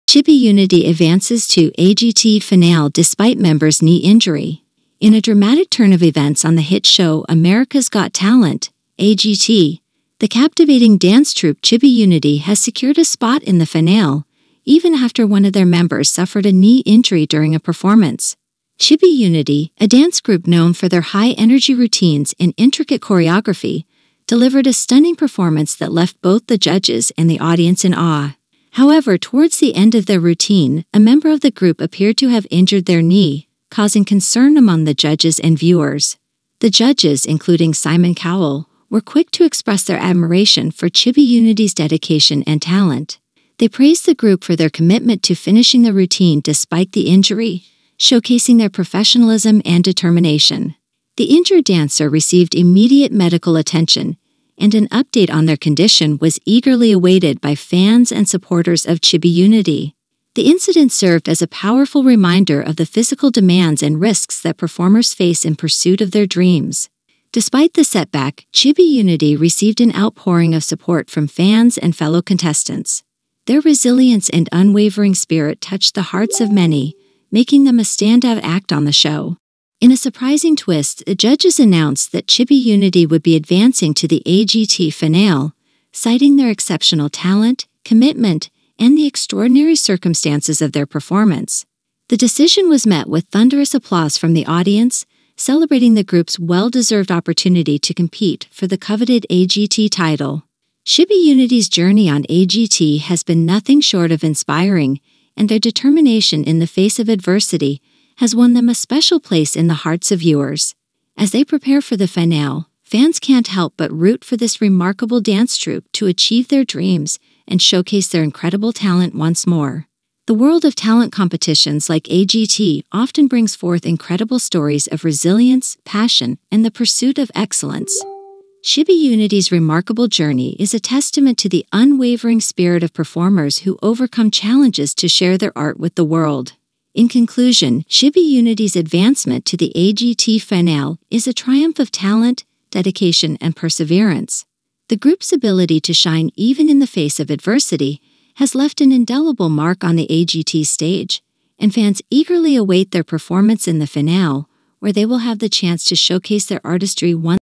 Golden Buzzer dance troupe Chibi Unity advances to 'AGT' finale after member injures knee
In a remarkable turn of events, the talented dance troupe Chibi Unity on 'America's Got Talent' has secured a spot in the finale despite facing a setback when one of their members suffered a knee injury. Their determination and resilience in the face of adversity have garnered widespread admiration, making them a fan favorite on the show. As they prepare for the AGT finale, viewers eagerly anticipate their next performance, which promises to be a testament to the power of passion and teamwork.